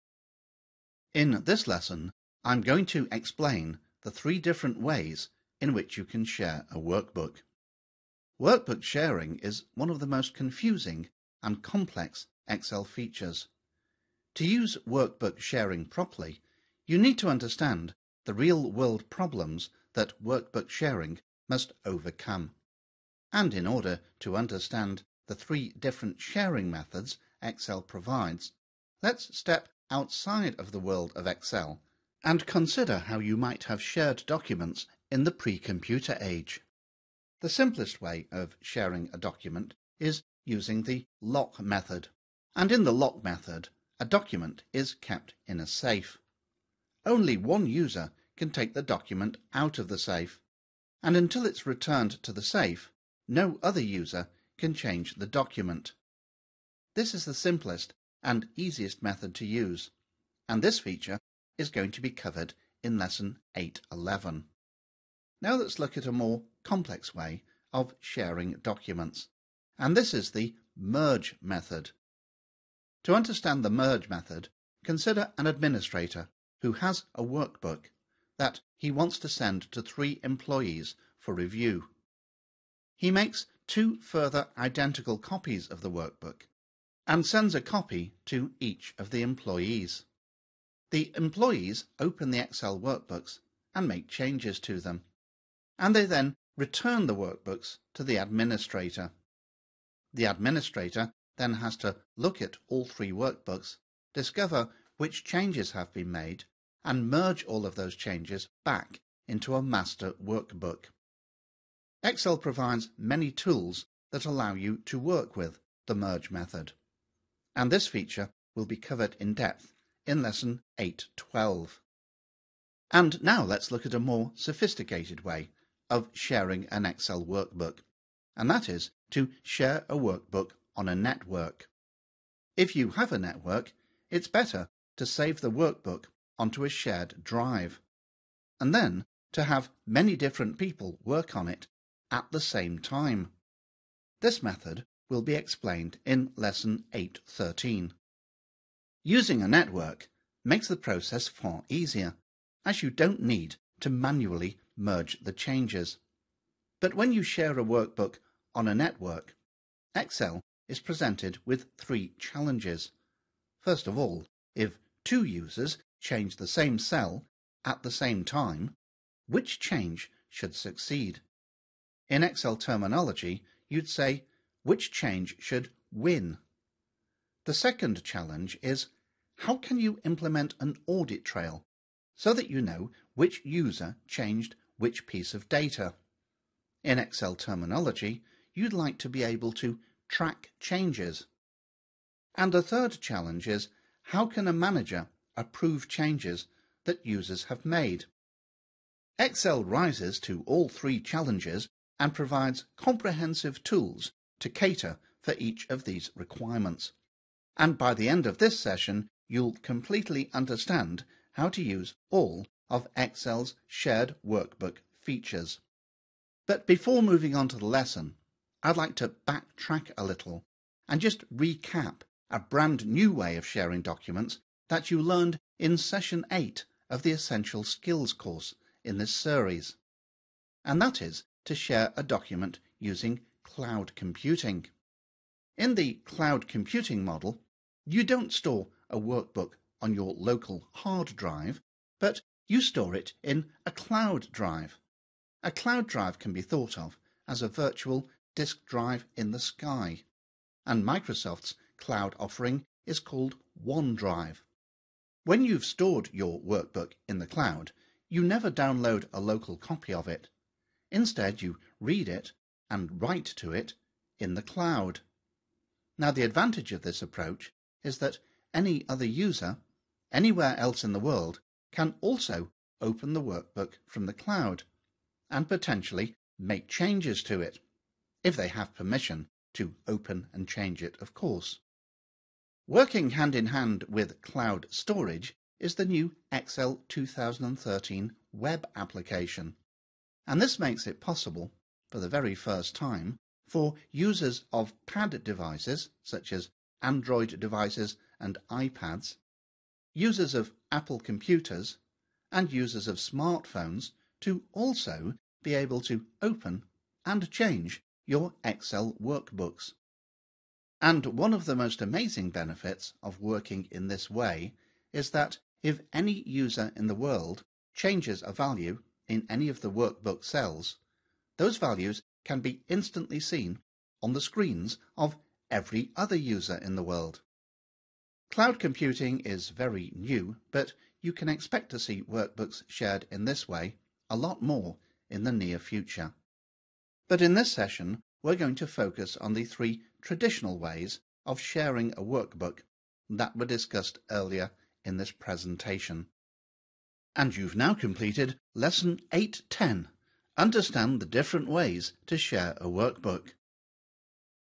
Excel Expert Lesson 8-10: Understand the three different ways to share a workbook
This video lesson provides a complete introduction to sharing in Excel.

In this lesson you'll learn about the three different methods of sharing an Excel workbook, including the Lock method, the Merge method and sharing workbooks on a network. This lesson explains the advantages and disadvantages of each method and also introduces a new method of sharing by using the Excel Web App.

This video comes from the Internet, Objects and Workgroups session (Session 8 in our Excel 2013 Expert Skills free video training course).